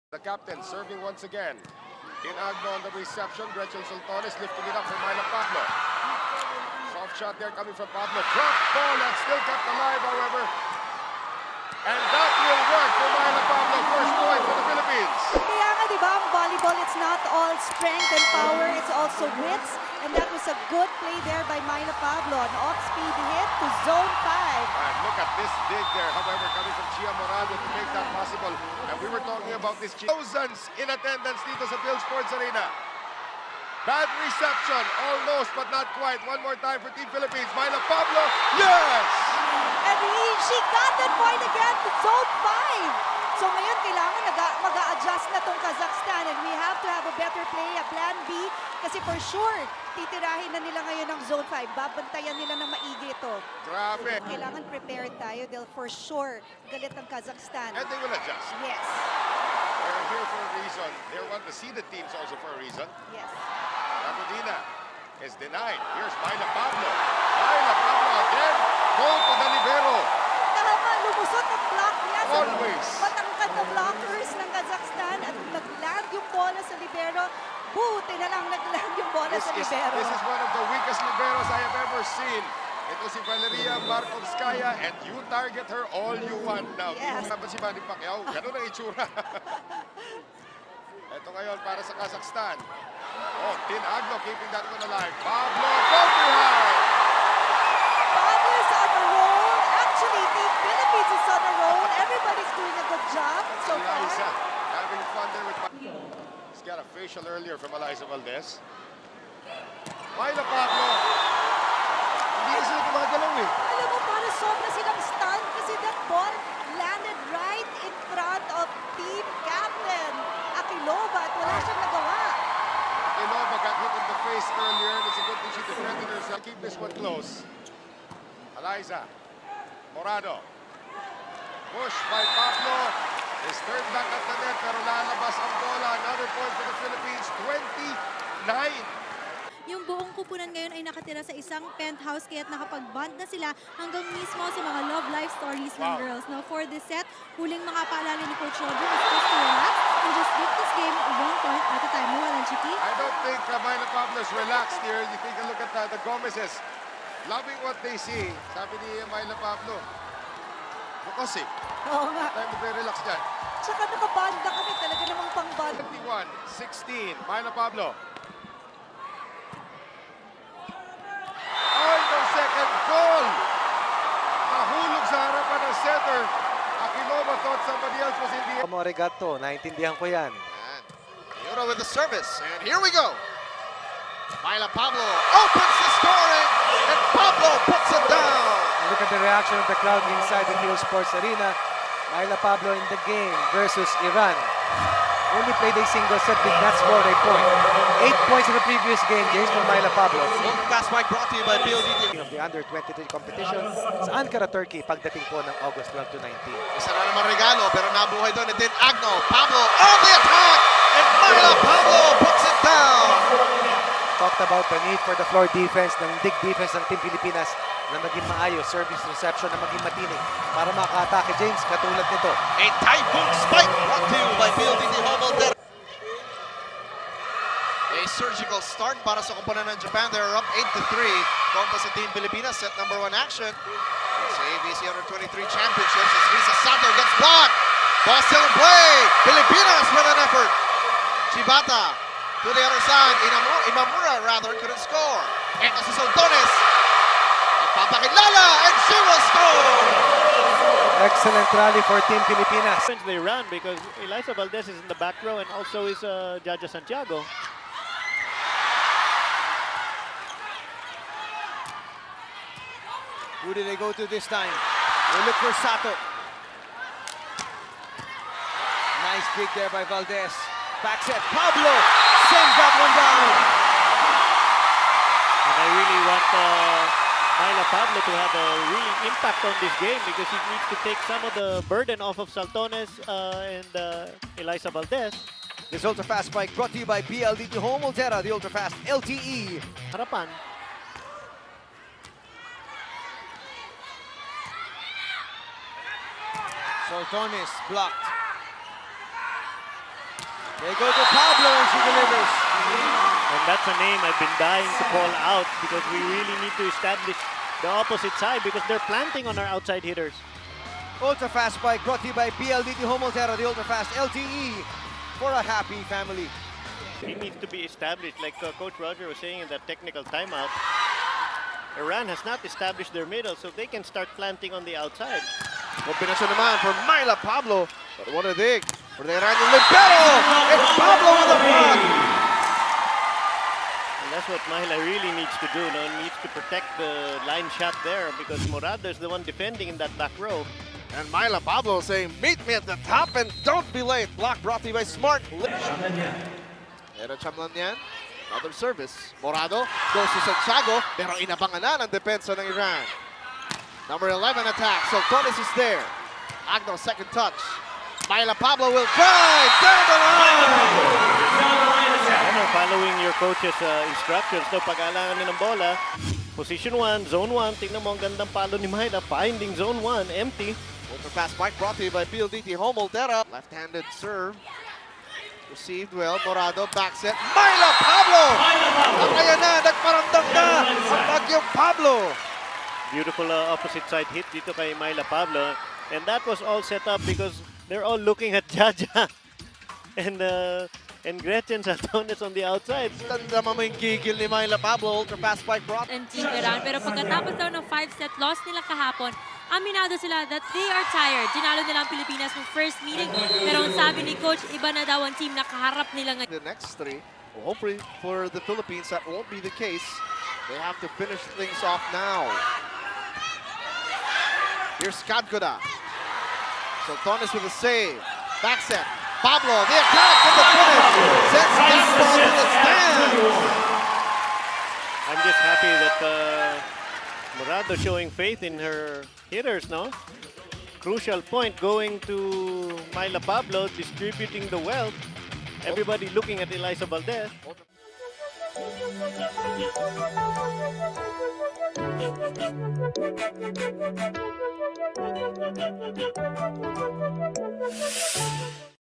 Myla Pablo | 2015 Asian Women's U23 Volleyball Championship | Compilation
2015 Asian Women's U23 Volleyball Championship journey of Filipino Opposite Spiker Myla Pablo (Philippine Women's Volleyball National Team)